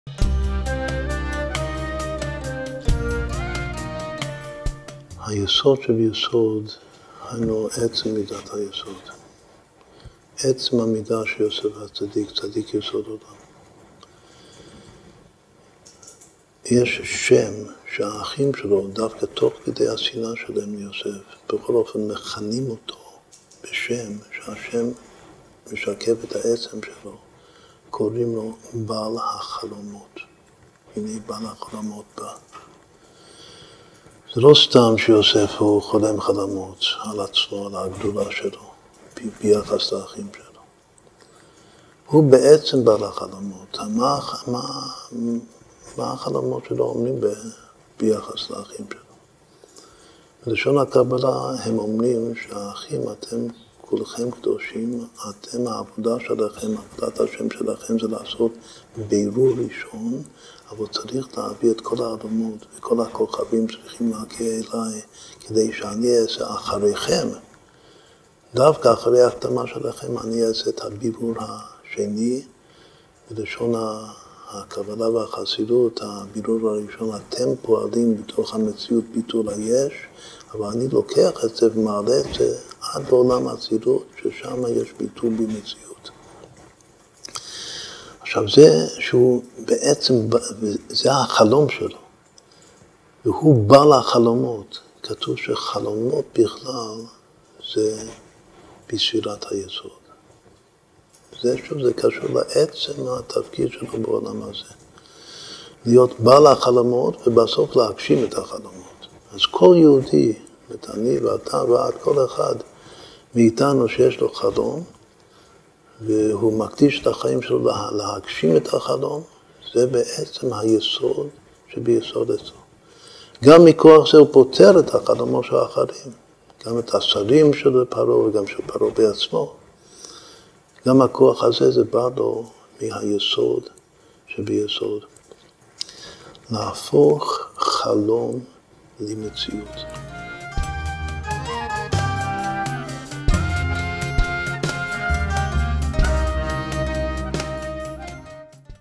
Ginzburg Yitzchak | שבעת הרועים - יסוד שביסוד – בעל החלומות
Ginzburg Yitzchak : האחים מכנים את יוסף בלעג "בַּעַל הַחֲלֹמוֹת הַלָּזֶה". נכון, יוסף הוא בעל חלומות, חולם בעצמו ופותר חלומות של אחרים. חלומותיו מראים לו את תפקידו ויעודו: להאיר ולמלוך, וגם לקחת את היבול הרוחני של האחים (האלומות) ולהעלות אותו לדרגה גבוהה יותר. החלומות מבטאים את הדחף של יוסף להשפיע – תכונת היסוד – ולכן זהו היסוד שביסוד, העצמיות של יוסף הצדיק-משפיע.
יוסף חי לאור החלום, הוא מקדיש את חייו להגשים ולאמת את חלומותיו-שלו, ומנווט את החלומות האחרים הבאים לפניו שיתאימו עם חלומותיו. תחלמו טוב, לא חלומות באויר אלא חלום פועל ומשפיע, חלום שהופך למציאות.

ניתן לצפות בשיעורים נוספים, להוריד קבצי שמע ותמלולים באתר מלכות ישראל:
ניתן לקבל סרטונים לוואטספ במספר - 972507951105+